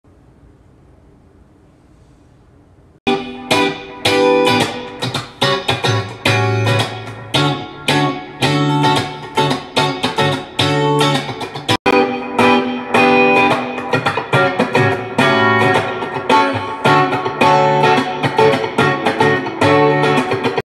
REAL Strandberg Vs FAKE Strandberg
Real Strandberg VS the NK Headless Guitar. This is an obvious copy of the strandberg and I am using the coil split on the NK Fishmans in comaprison with the single coils.